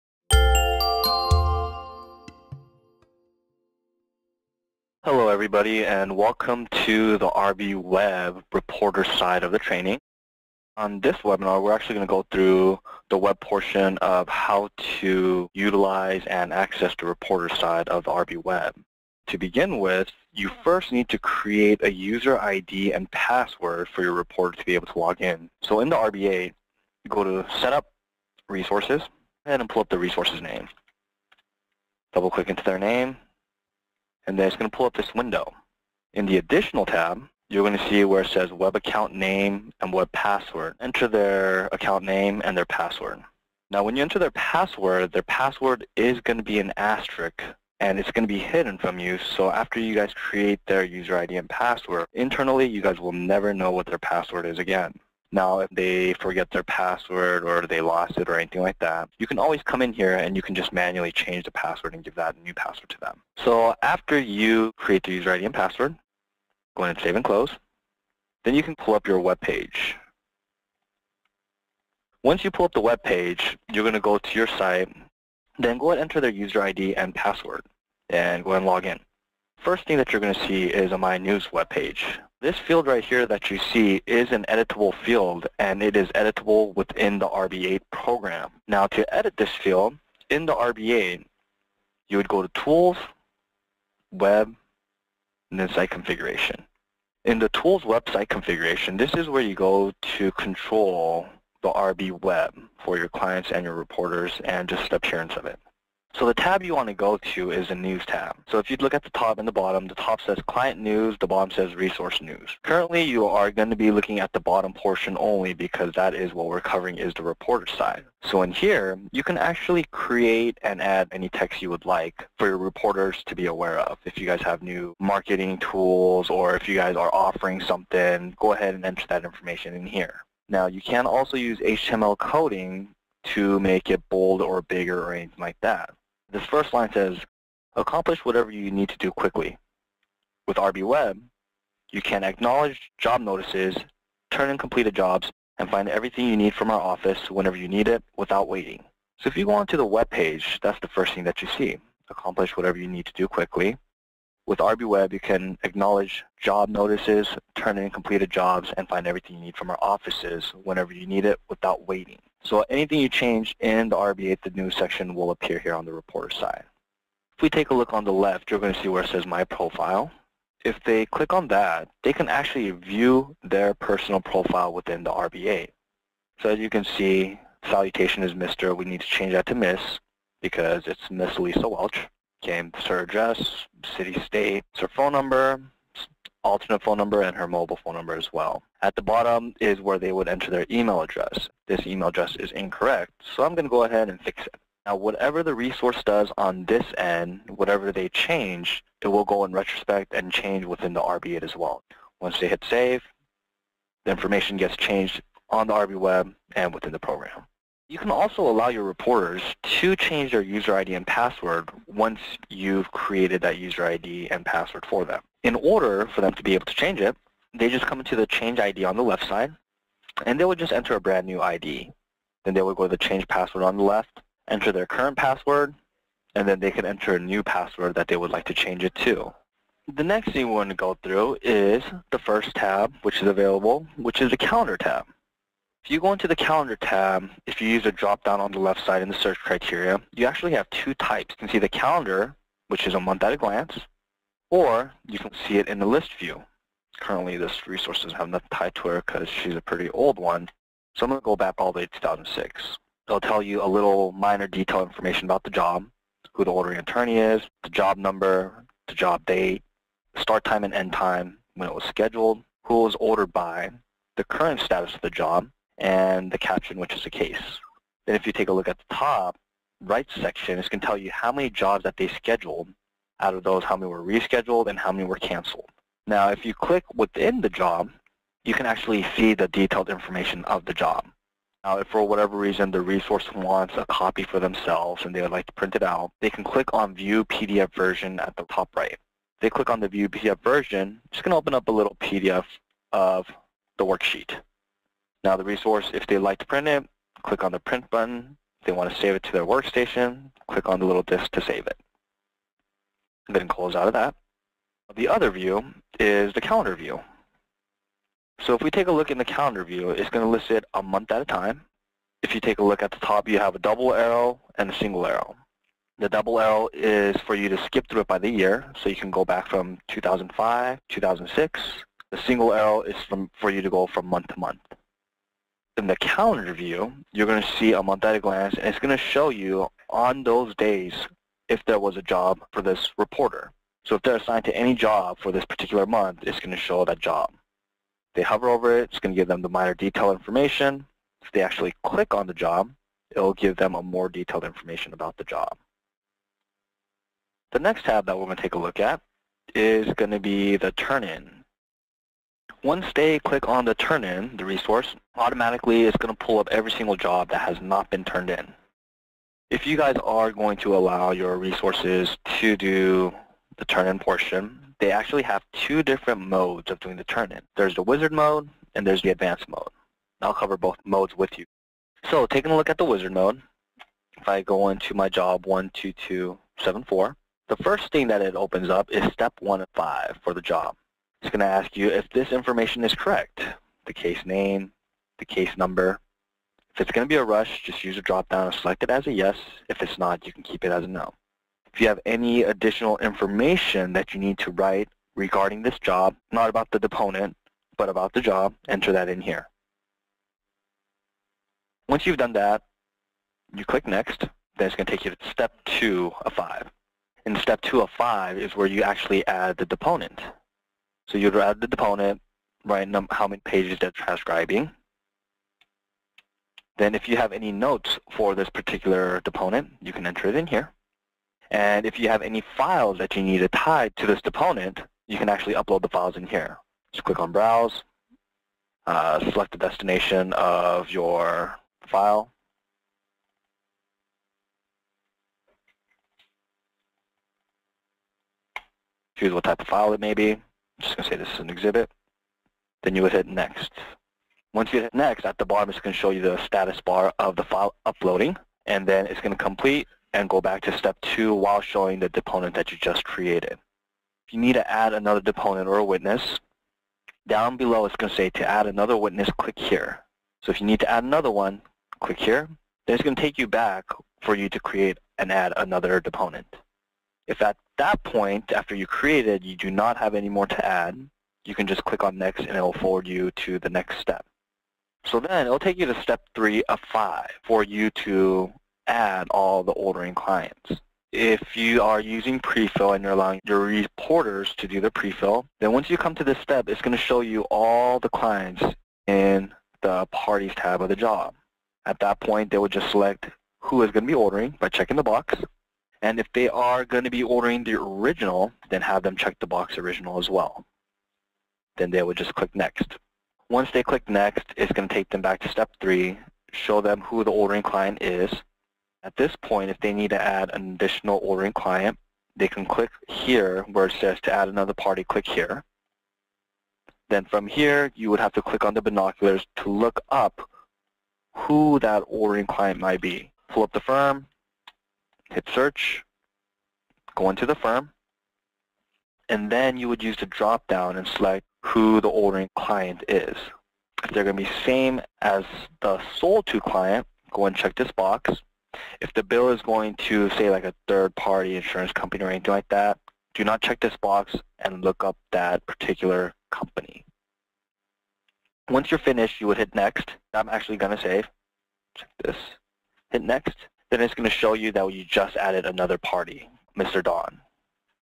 RB Web Reporter Training: Part 1
First installment in 3-part series of RB Web training for court reporters and other resources. Set up users, landing page content, user profile and change ID/password. Overview of Calendar and Online Turn-in wizard, steps 1-3.